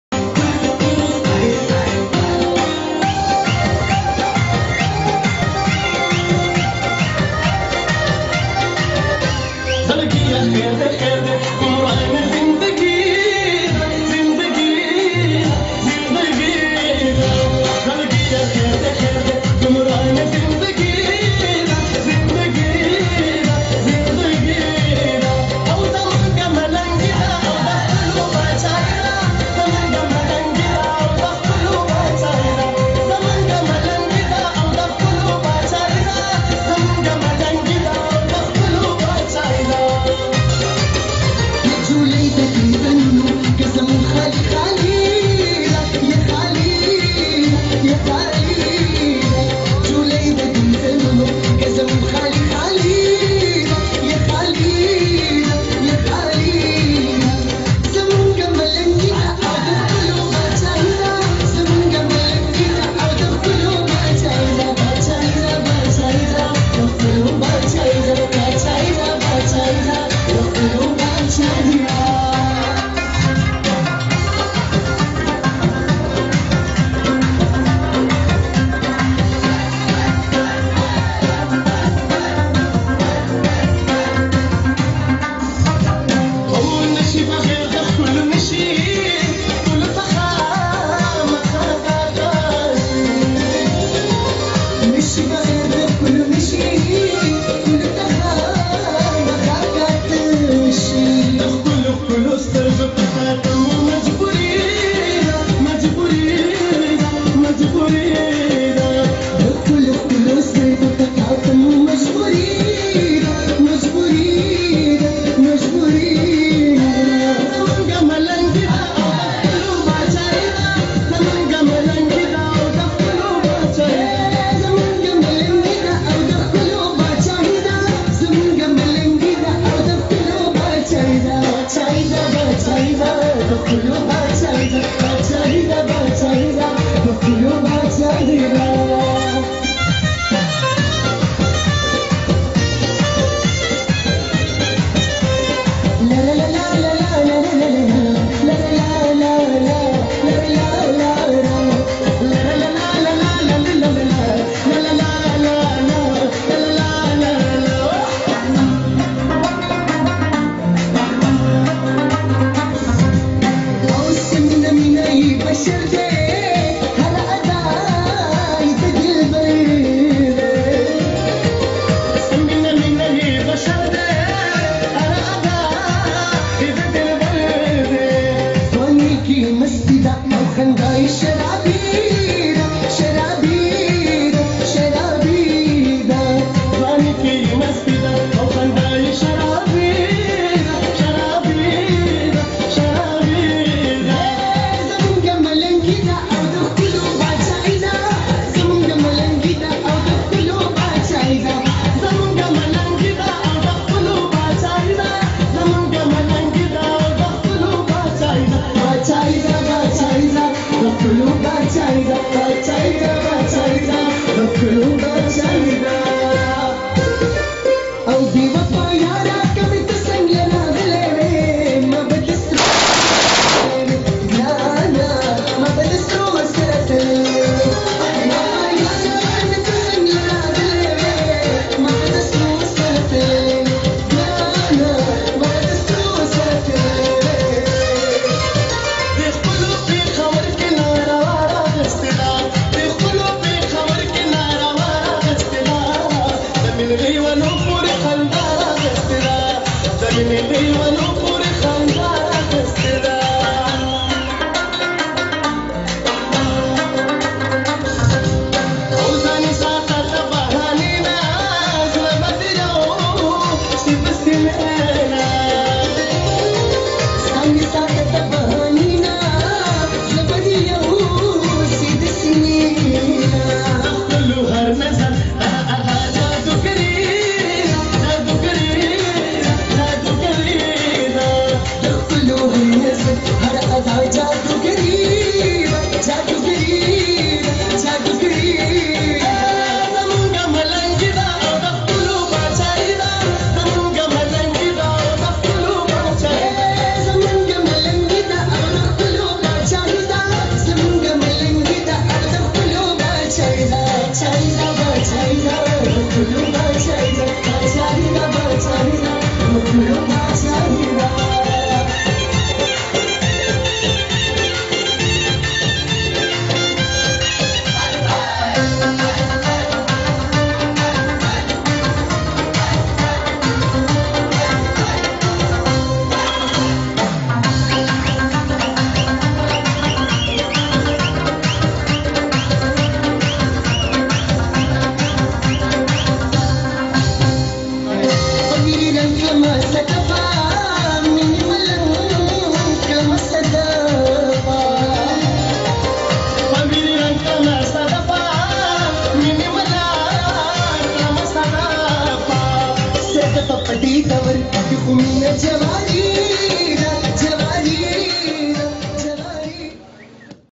Pashtoo Song Mahnoor Peshawar
Mahnoor From Peshawar
Dance Performance Wedding Function At Bhair Kund Mansehra